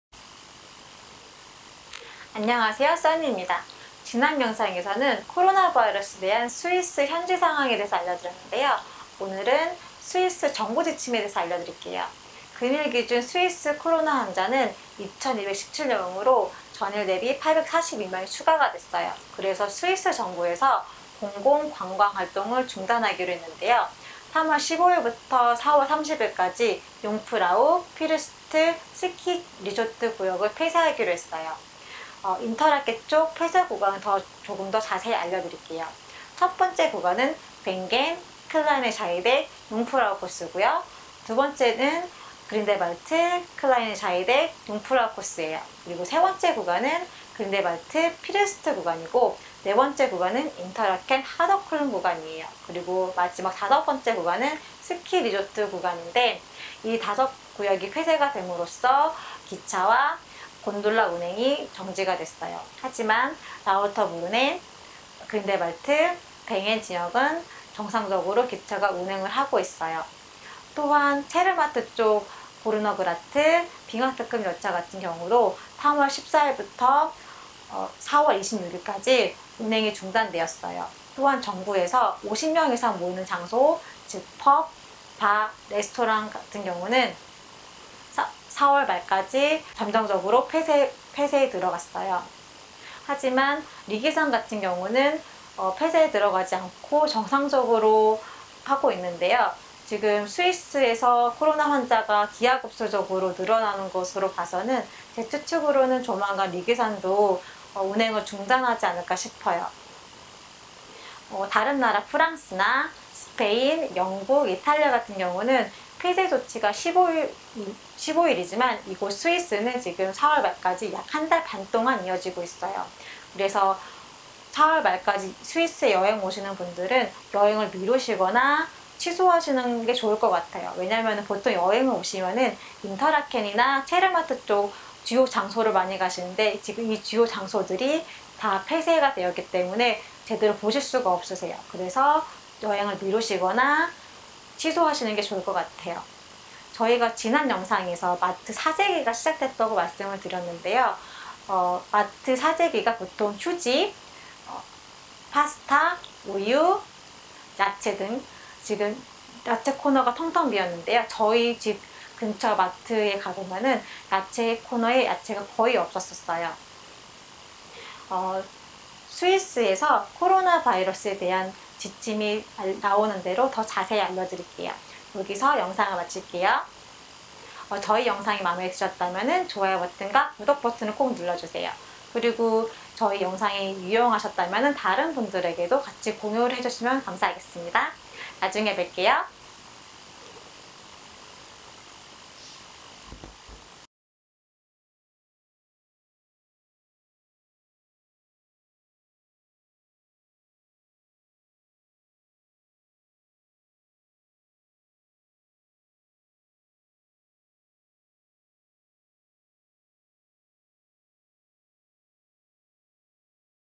[스위스 현지상황] 코로나바이러스 스위스 현지상황 | 융프라우 운행중지 | 고르너그라트 운행 중지 | 마트 사재기! [KOR/ENG/SPA SUB]
안녕하세요 국제커플로 스위스에 살고 있는 한국여자 써니 & 스페인 남자 제리 입니다. 스위스 베른 현지 가이드인 제가 스위스 코로나 바이러스 현지 상황에 대해 알려 드릴께요.

이탈리아를 시작으로 유럽에 코로나 바이러스 확진자가 많아 졌는데요. 또한 스위스 현지상황도 계속해서안좋아지고 있어요. 스위스에도 코로나 바이러스 확진자가 많이 늘고 난 후, 스위스 정부에서 융프라우 지역 및 체르마트 지역 운행 중지를 내렸는데요. 또한 50인 이상 모이는 장소(펍, 레스토랑)등도 잠정 폐쇄 되었어요.
언제까지 융프라우와 체르마트 지역이 운행 중지 알려드릴께요.
계속해서 스위스 코로나 바이러스 현지상황 자세히 알려 드릴께요.

더 좋은 영상으로 찾아 뵙겠습니다.

베른 가이드 문의는 인스타그램 DM으로 문의주세요^^
코로나바이러스 스위스